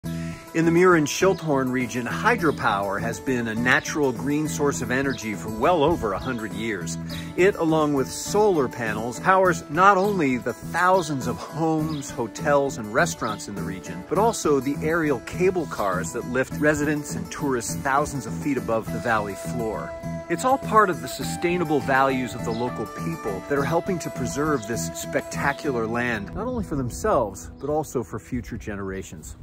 Jeff Wilson visits the Swisstainable Schilthorn | Switzerland Tourism
In Switzerland we've always cared deeply for nature. Sustainability is in our DNA. Learn more about how we preserve Switzerland as a sustainable travel destination: